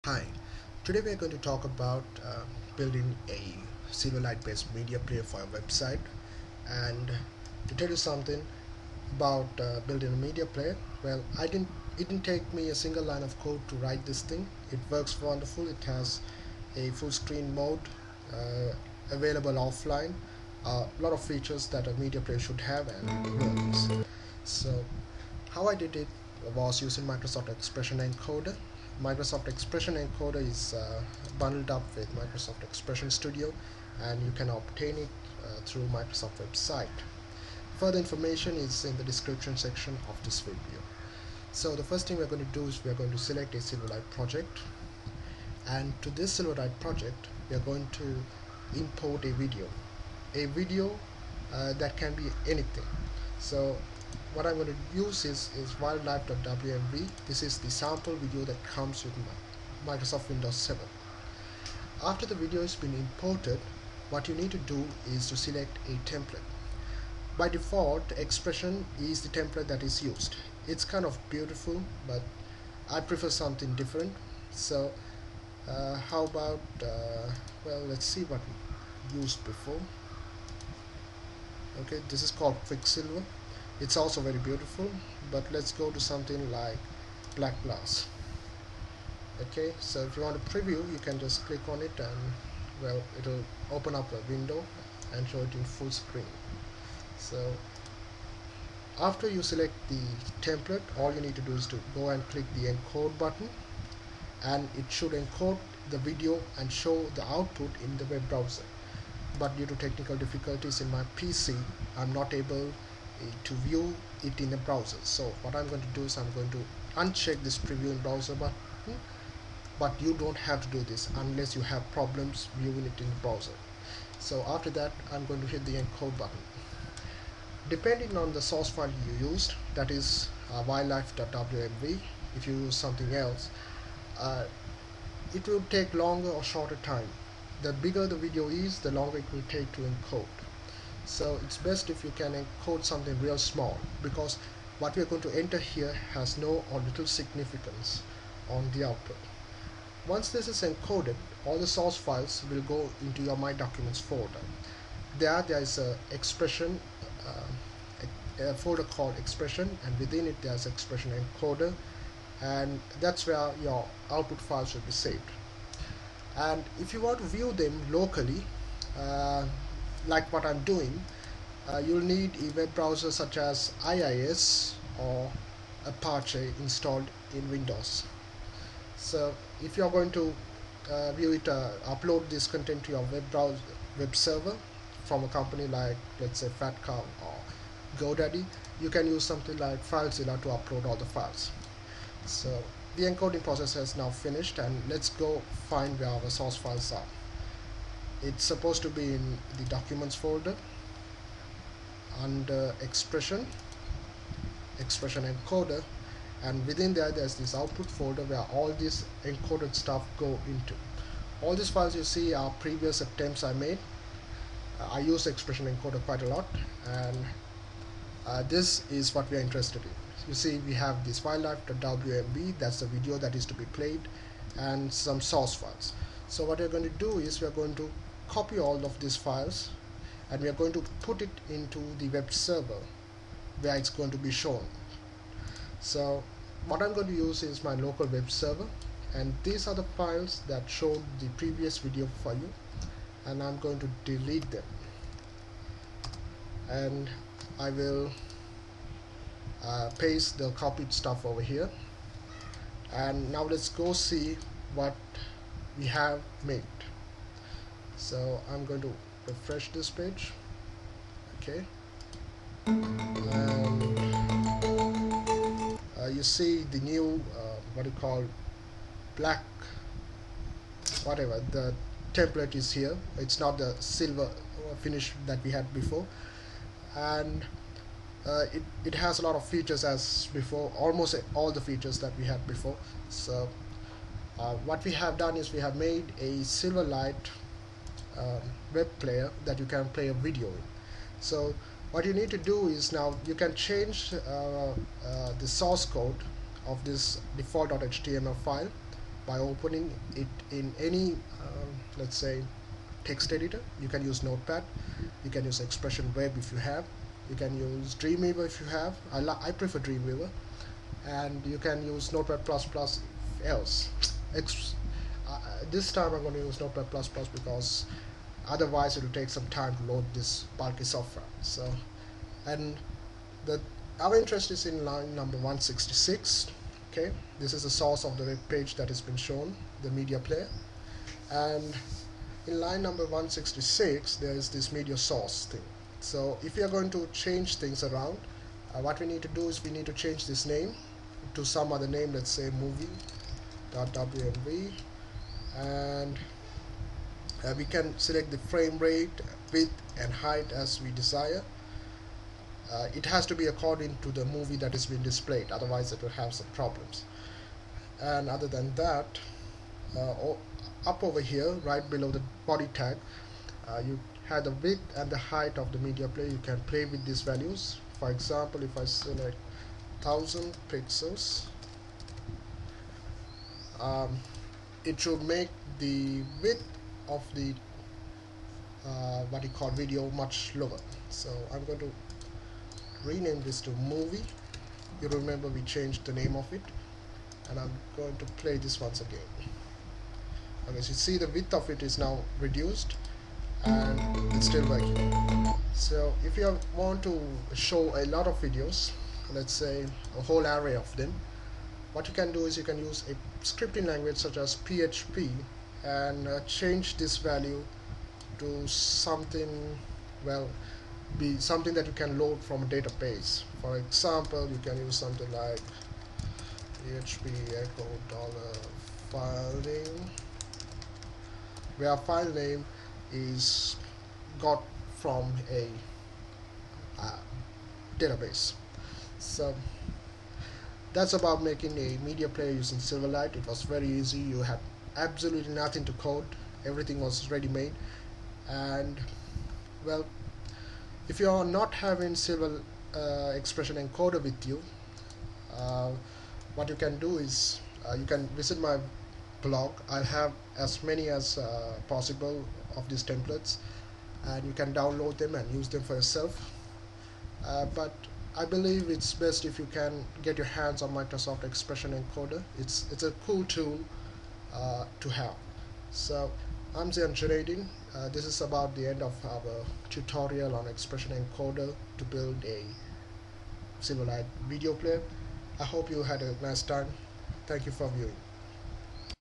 Silverlight Media Player For Webmasters - Expression Encoder 4
Silverlight is technology similar to Adobe Flash and is produced by Microsoft. The technology is currently used to build the user interface of Windows Phone 7. Although Silverlight is not widely used as much as Flash is, it is fair to say it is really cool environment to develop.

Today we discuss how you could use Microsoft Expression Encoder 4 Pro, bundled along Expression Design and Expression Web in to a package Microsoft Expression Design, to build a Silverlight media player with absolutely no use of programming. All dummies editions will include programming and I am wondering  if it is right to call this 'Dummies Guide to Instant Silverlight Media Player Development' ;)

Software Used:
01. Microsoft Expression Encoder 4 Pro
02. IIS or Apache for local hosting else a web hosting account.
03. Internet Explorer or any other browser

Basic Steps:
Open Expression Encoder, import any video, select template from the template tab and encode. Go to the Expression encoder output folder, copy the source files to your web-server and view it in Internet Explorer.

Open default.html file and change the MediaSource to the desired file name and do any changes to the HTML to customize the media players size and position. Save and view again.

Thats it.

Ziyan Junaideen